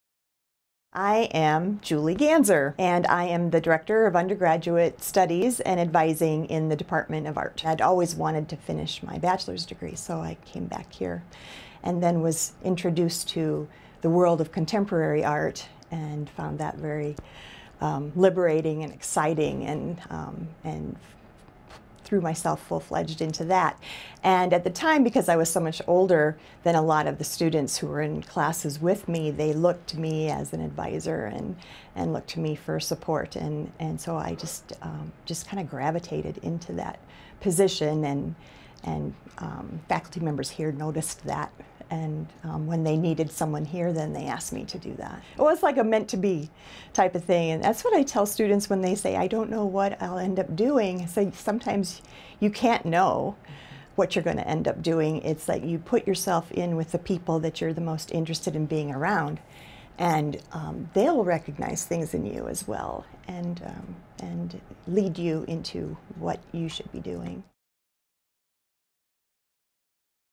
Julie Ganser: UW-Madison Director of Undergraduate Studies and Advising| UW School of Ed | Art Dept.
University of Wisconsin–Madison School of Education. Art Department.

Learn more about Julie Ganser, the UW-Madison art department advisor. Listen to her talk about her natural gravitation towards advising and her love for what she does.